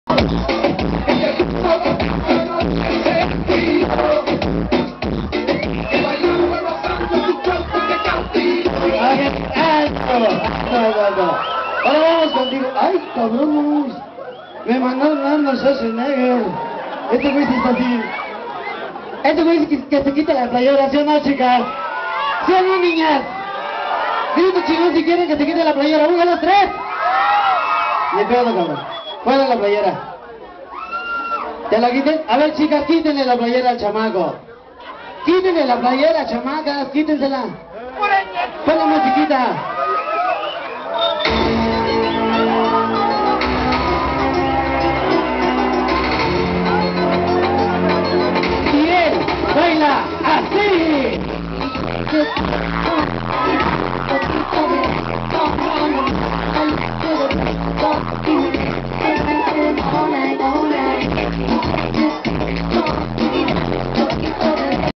SONIDO NEMESIS EN VIVO LOCUCION ANGELO'S Y DJ ULISES DIRECCION GENERAL FERNANDO BRIZUELA
AKI LES DEJAMOS ESTE VIDEO DE SONIDO NEMESIS EN VIVO PARA KE VEAN NUESTRO AMBIENTE